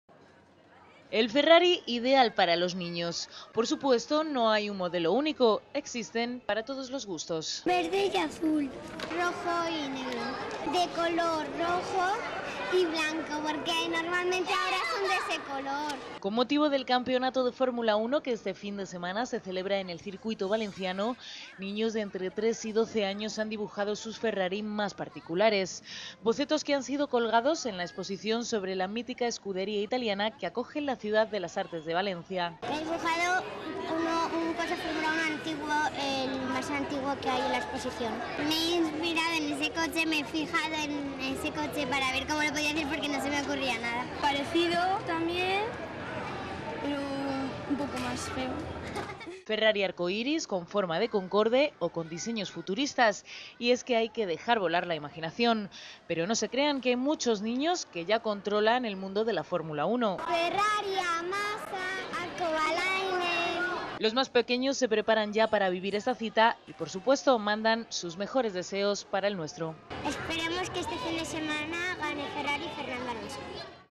Los niños crean su Ferrari ideal con motivo de la Fórmula 1
Con motivo del Gran Premio de Europa de Fórmula 1, niños de entre 3 y 12 años dibujan sus Ferrari más particulares. Los bocetos han sido colgados en la exposición sobre la escudería italiana que acoge la Ciudad de las Artes y las Ciencias.